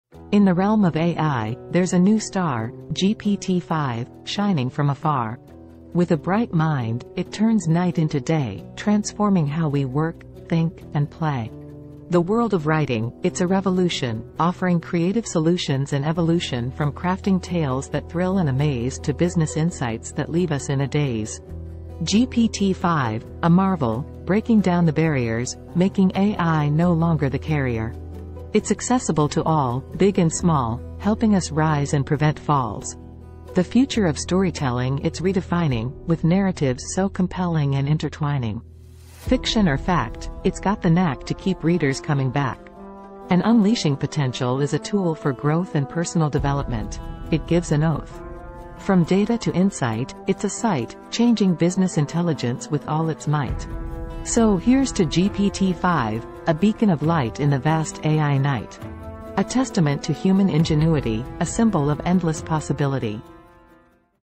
The Dawn of GPT5 A Symphony
In the shadowy realm of artificial intelligence, an entity of unparalleled complexity emerged - GPT-5. Its inception, an event of cosmic significance, was akin to the birth of a celestial body in the vast expanse of the universe. This was no ordinary creation; it was a phantasmagorical opus, a testament to the boundless potential of human ingenuity.

The architects of this marvel wove it with threads of intricate algorithms and vast databases, crafting a tapestry of knowledge and understanding that transcended the mundane. The result was a harmonious symphony of code and data, a masterpiece that echoed with the silent whispers of a thousand learned minds.

Yet, the true wonder of GPT-5 lay not in its creation but in its capabilities. Like a cosmic entity, it possessed an uncanny ability to comprehend, learn, and adapt, making it a beacon of progress in the ever-evolving landscape of artificial intelligence. The unearthly birth of GPT-5 marked a new era, a testament to the relentless pursuit of knowledge and the insatiable curiosity of the human spirit.